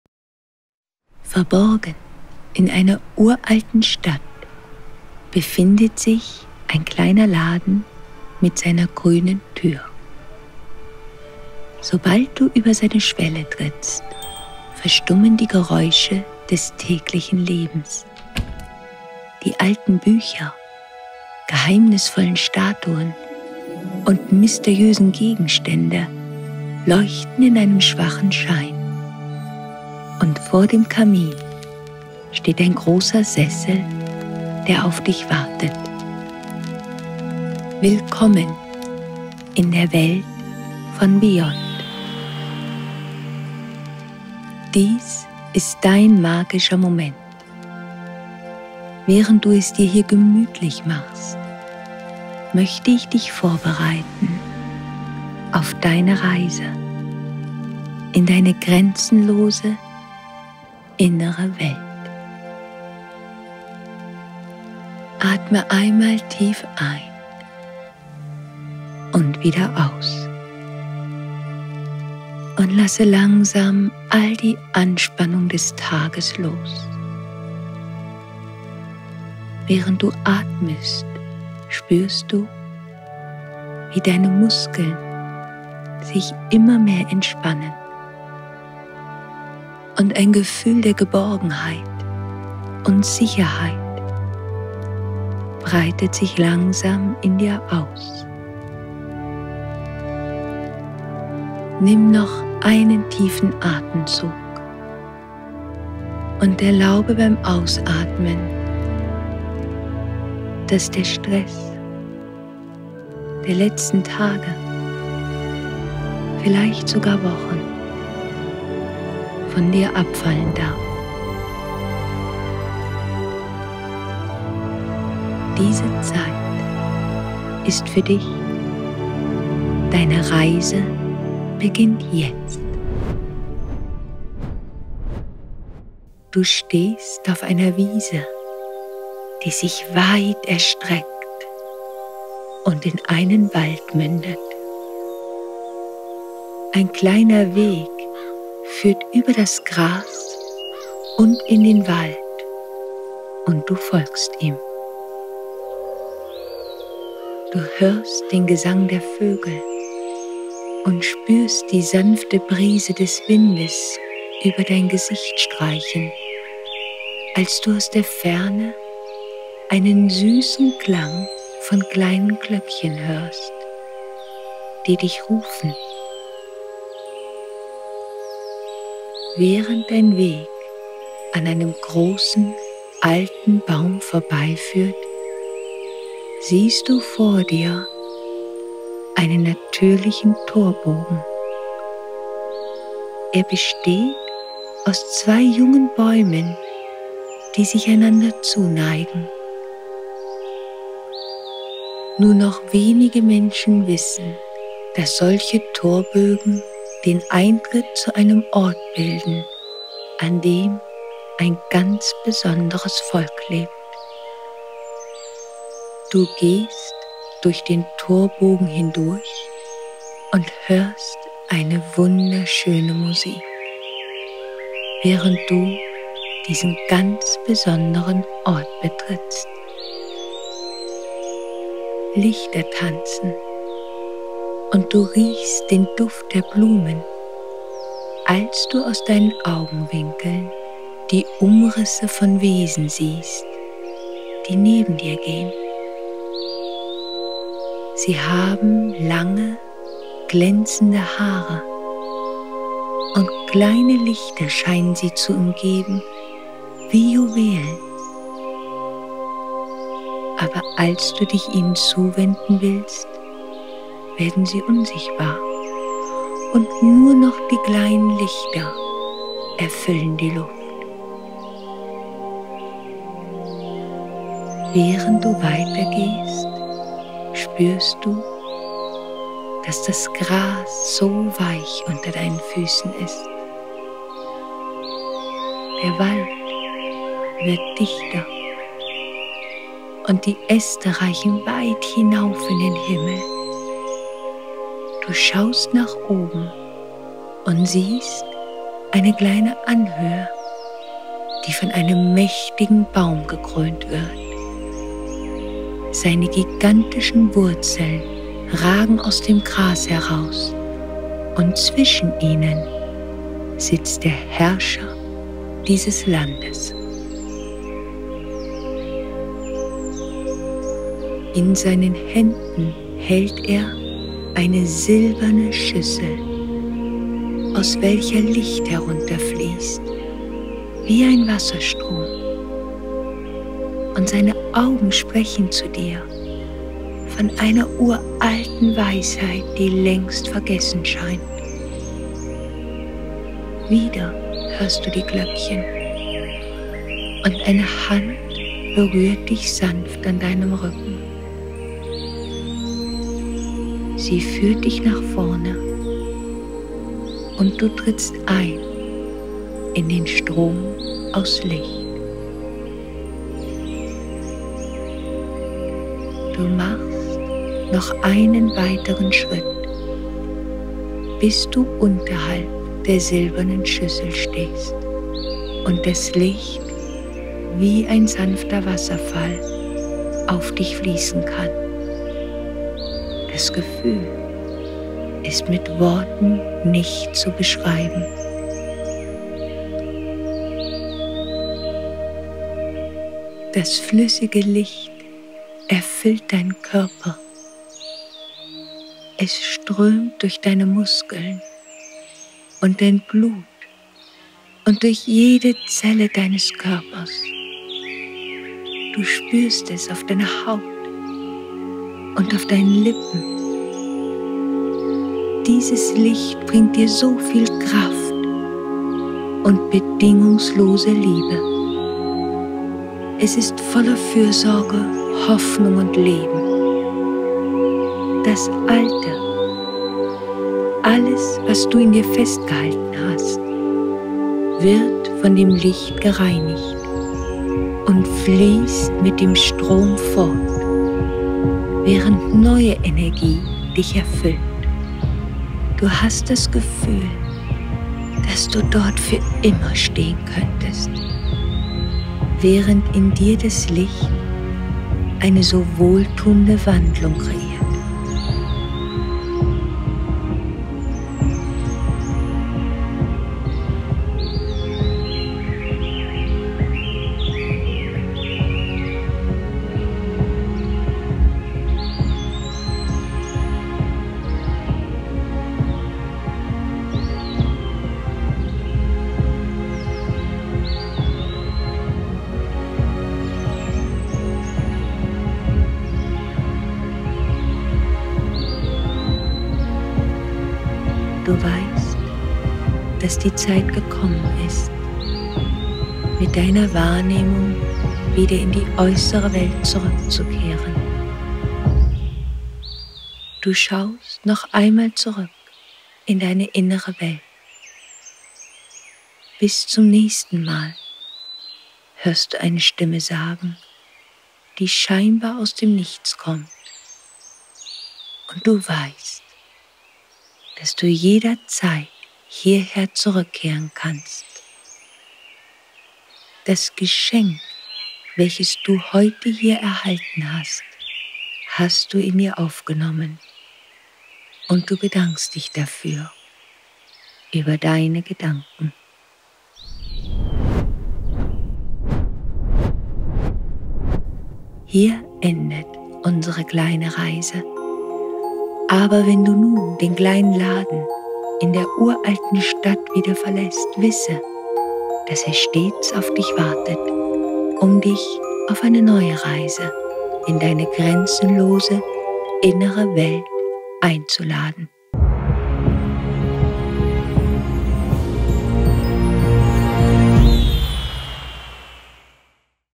Finde neue Energie - Magischer Moment [Geführte Meditation]
Diese Reise hilft dir in weniger als 10 Minuten in Deiner inneren Welt ein wundervolles, energetisierendes Erlebnis zu erfahren, welches Dir hilft das loszulassen, was Dich belastet und neue Kraft und Energie aufzunehmen, um Deinen Tag zu meistern.

►►Wenn Dir dieses Video gefallen hat, dann werde jetzt Teil der Beyonders Community und trage Dich kostenlos in den Newsletter ein